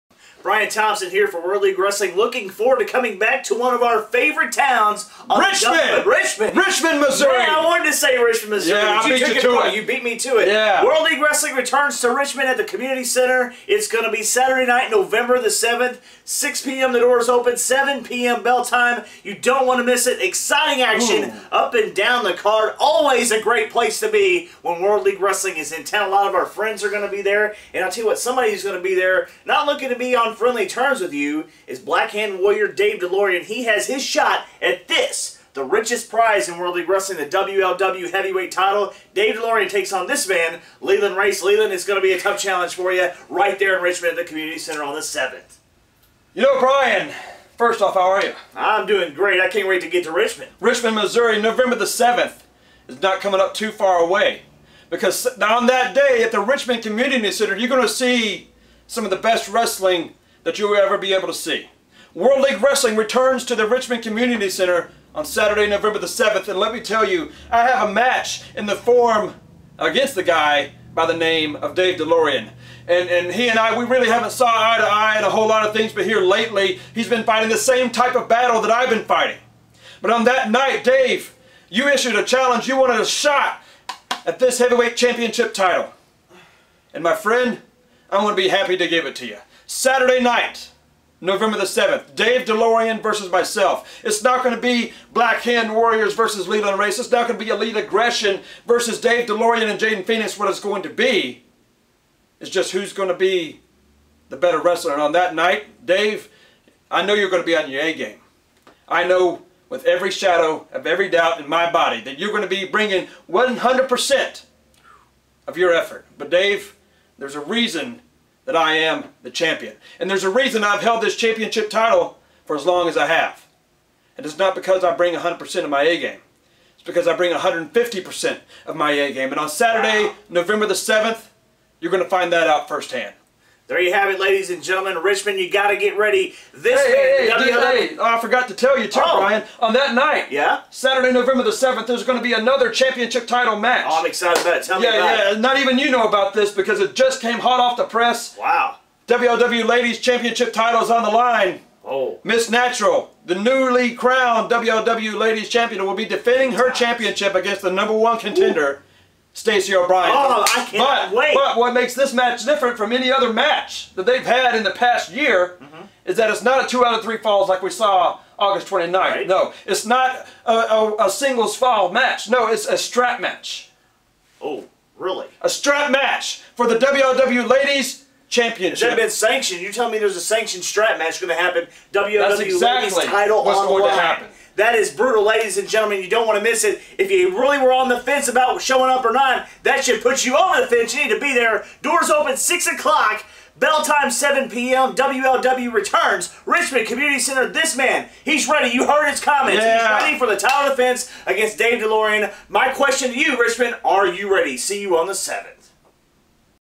November 7th in Richmond, MO
WLW Champion, Leland Race, discusses WLW's upcoming event in Richmond, MO at the Richmond Community Center on November 7th, 2015!